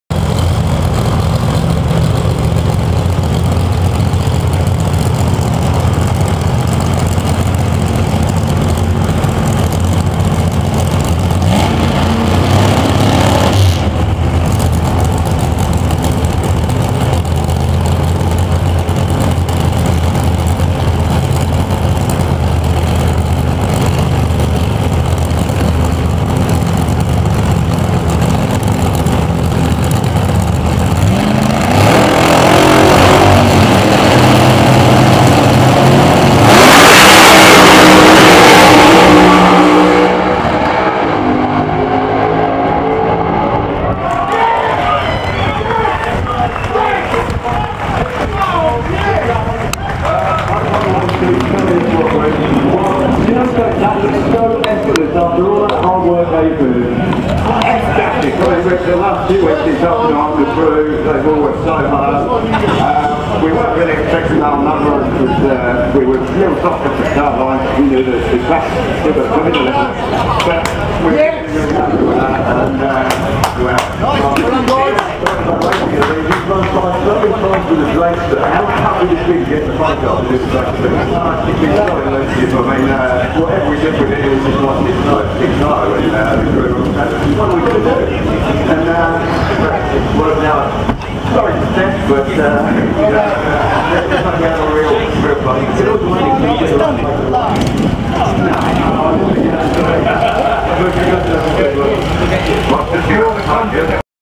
Turner MS Record Run
Rob Turner & Turner Motorsport become the 1st British crew to run a sub-6 second quarter mile at Santa Pod, September 2011. Crew Chief Steve Turner is interviewed after the run as the crew celebrate.